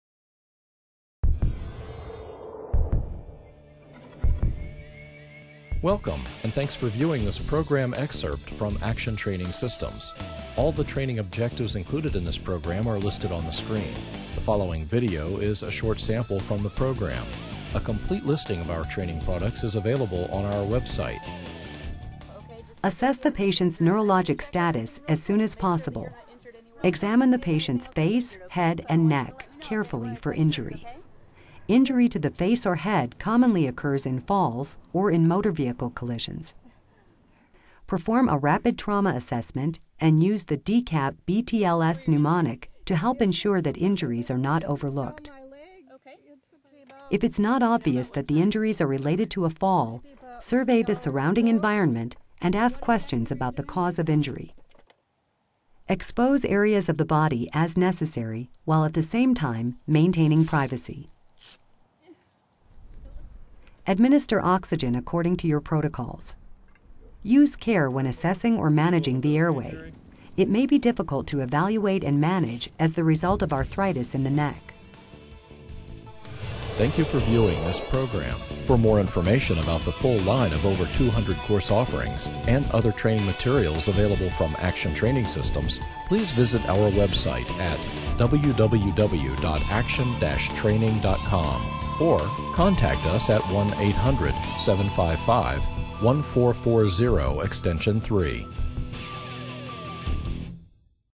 EMT Training (21) Geriatric Emergencies by Action Training Systems
Presents emergencies particular to the geriatric patient. Describes age-associated changes in geriatric patient systems and how care approaches must be adjusted when responding to and caring for an elderly patient. Demonstrates how to assess and provide emergency care for geriatric medical and trauma patients.